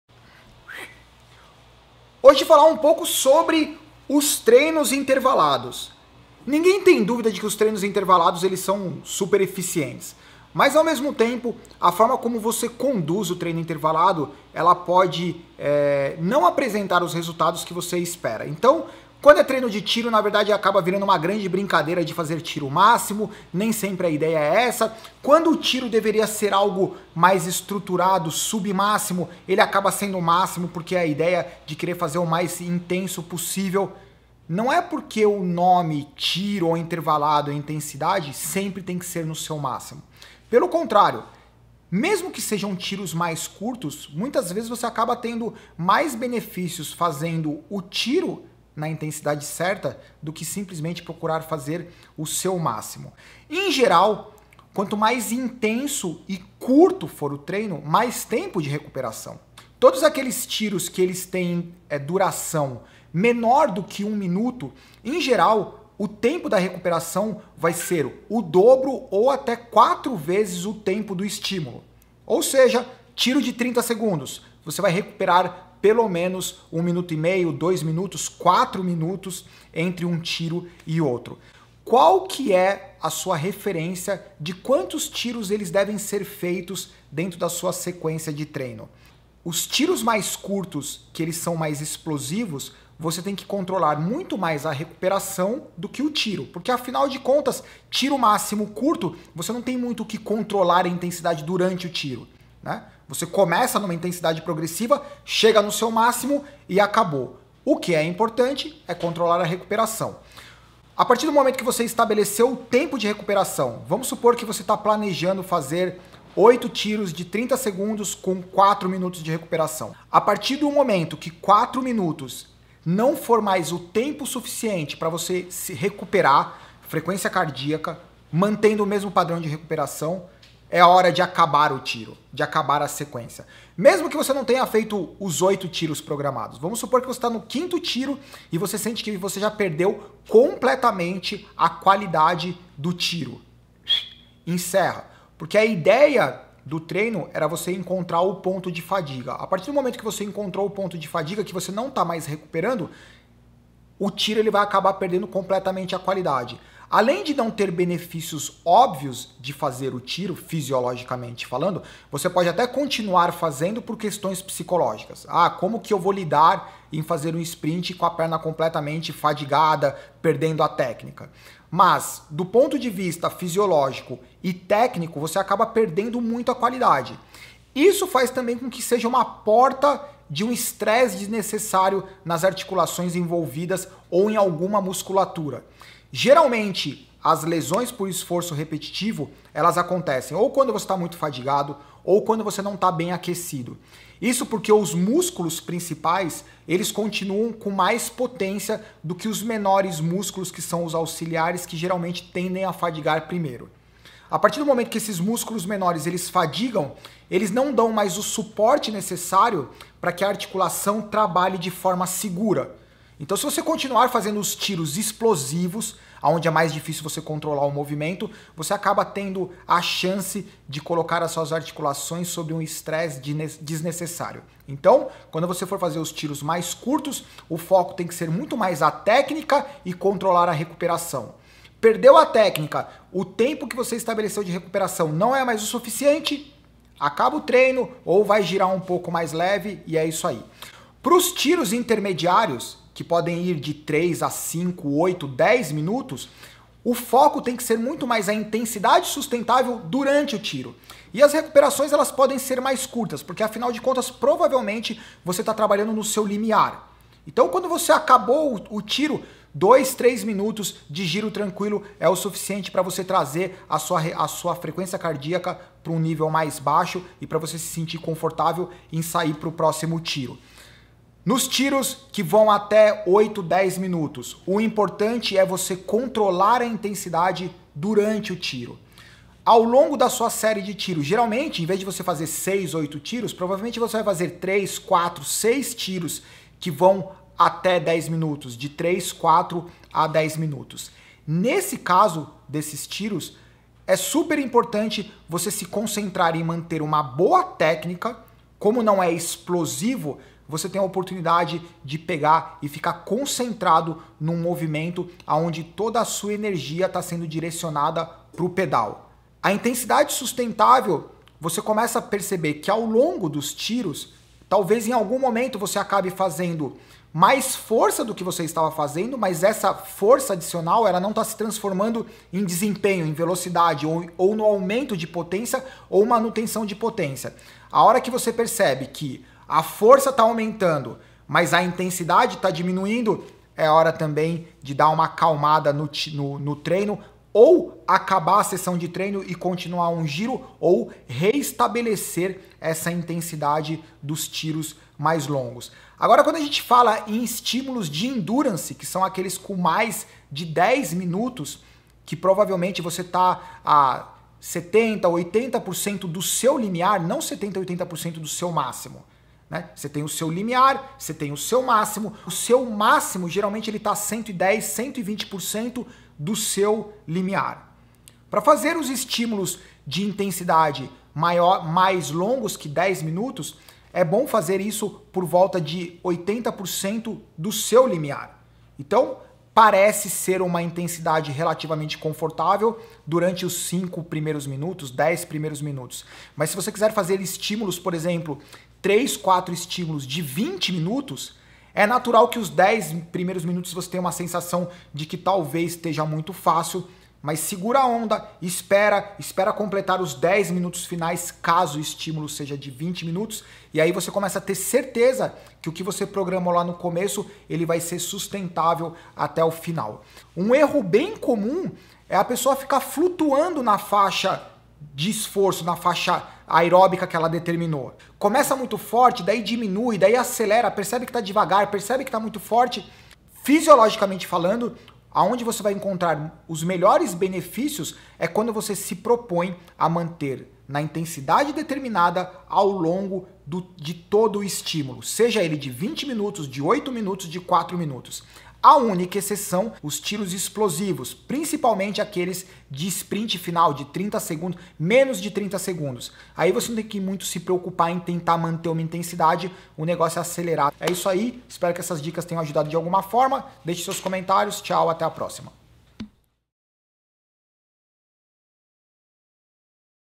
Treino Intervalado!!! Algumas Características.
Este vídeo é sobre Treino Intervalado!!! Sem dúvida é uma ótima opção para acelerar o processo de evolução dos treinos e fundamental para estar preparado para enfrentar diferentes situações com eficiência .